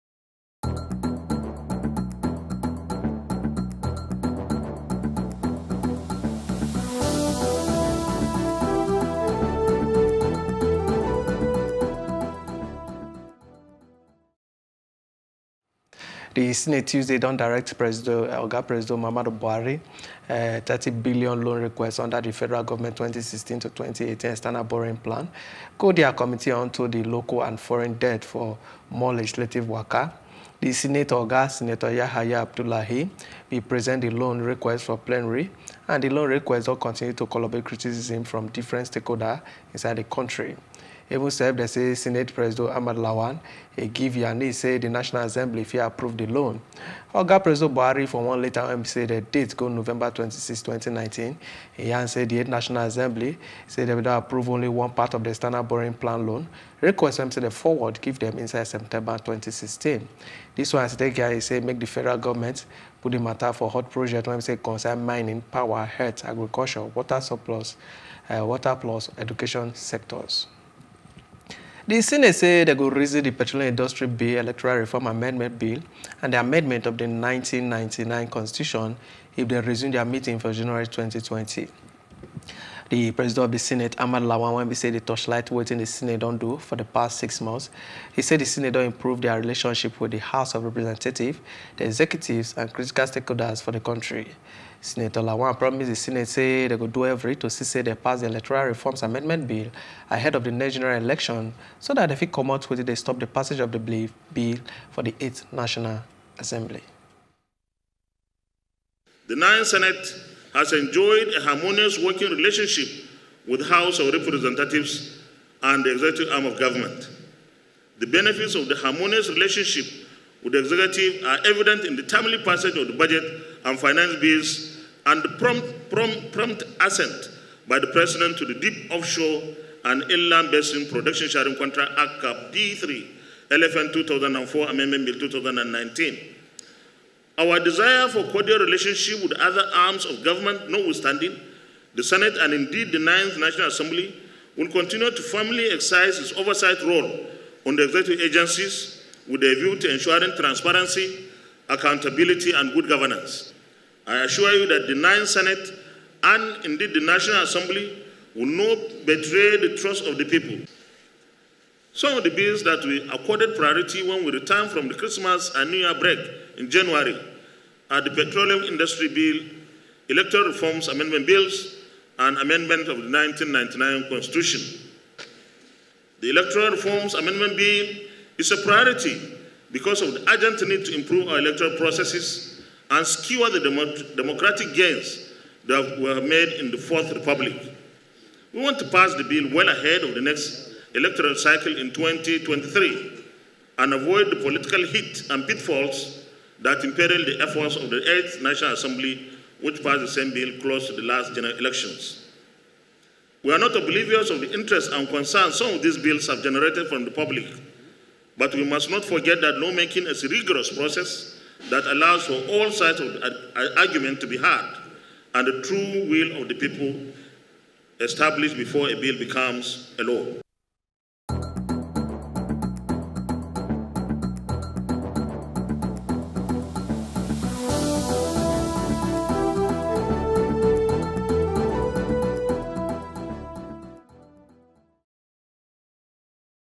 Senate Onto Electoral Reform Bill
The Senate say dem go reason de Petroleum Industry Bill, Electoral Reforms Amendment Bill and the Amendment of the Nineteen-ninety-nine Constitution if dem resume dia meeting for January twenty-twenty.